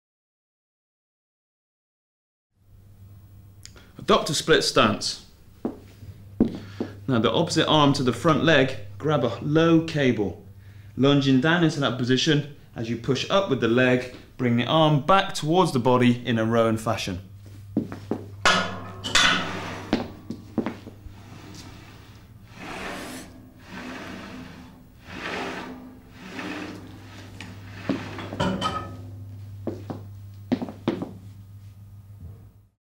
Split squat to one arm row exercise
Form guide:
Adopt a split stance, grab the cable with one hand at stomach height and lunge down. As you push up, bring the arm back towards the body in a rowing fashion.